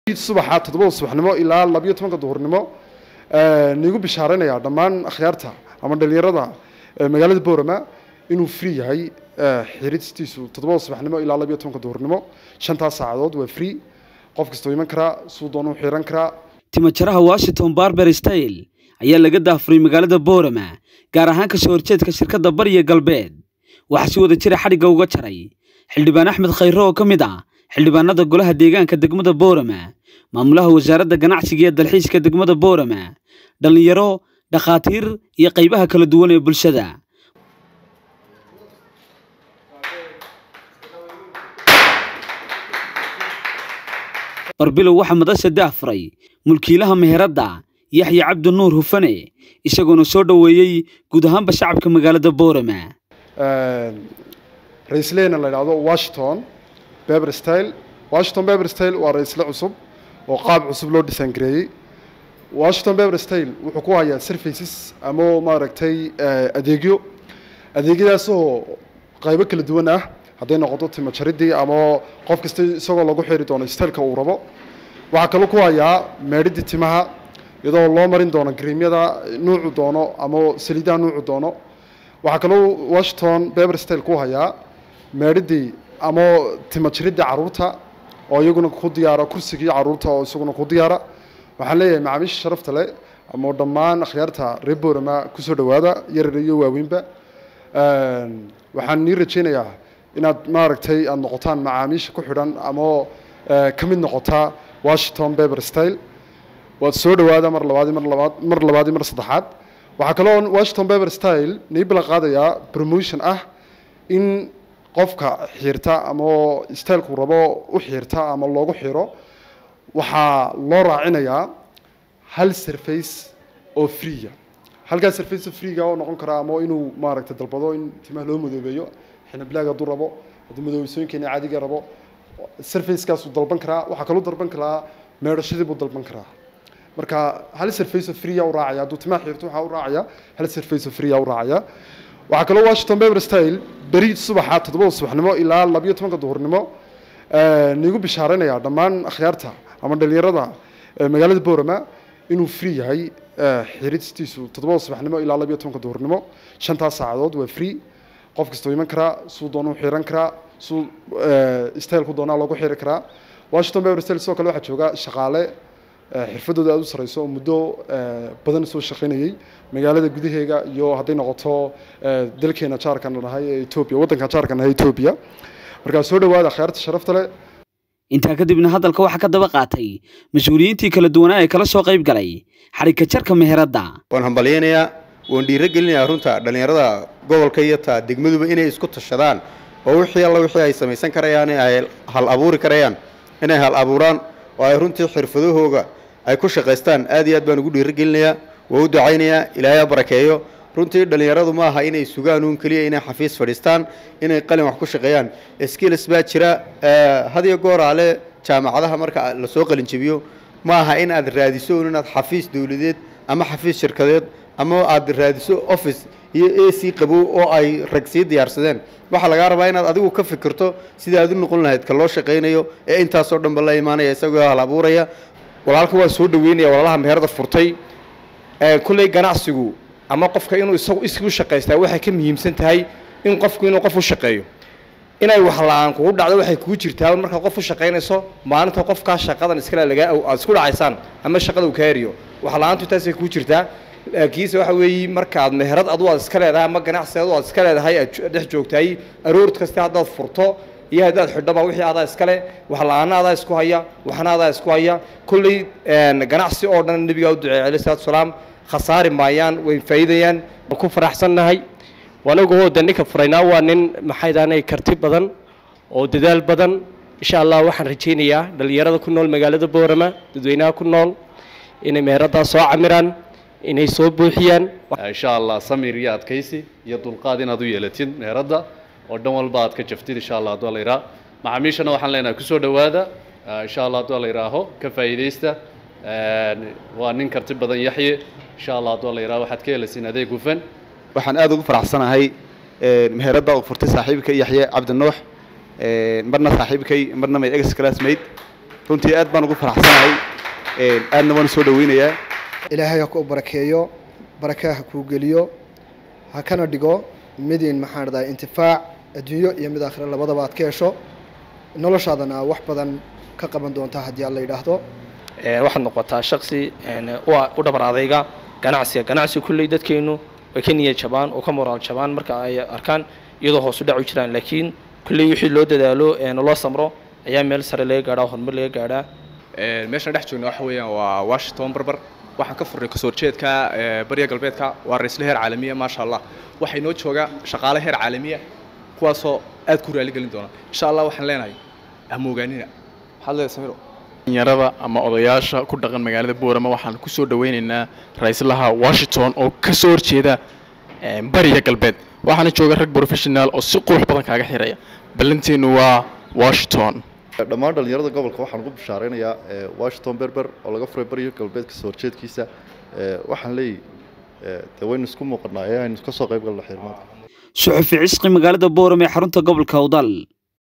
Bari 7-Subaxnimo ilaa 12 duhurnimo waa bilaash Xiiristu ka faa'iidaysta, Timajaraha Washinton Barber
Bari 7-Subaxnimo ilaa 12 duhurnimo waa bilaash Xiiristu ka faa'iidaysta, Timajaraha Washinton Bar-ber Style ayaa Laga Daahfuray Magalada Borama.

Suxufi Ishqi halkan uga yeedh sxbkaaga timuhu ku baxeen